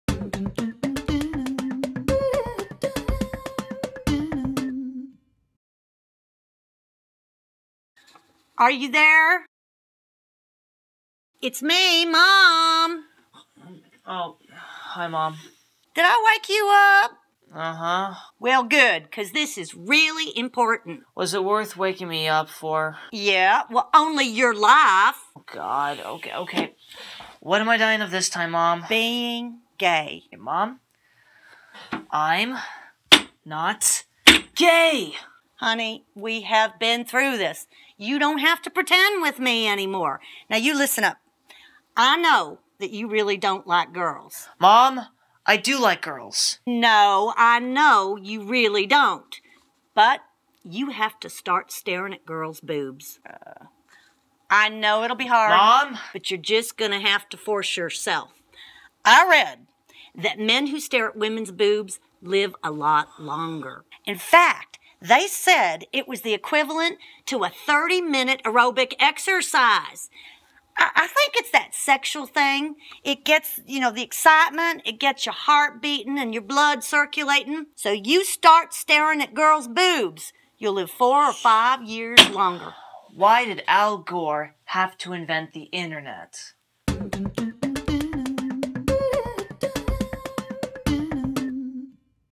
eMom 10 Boob Boy - Mom tells her son to look at more Breasts
eMom is at it again.  This time she is online video chatting with her son and telling him that he needs to look at lots of woman's breasts.  If she reads it she believes it.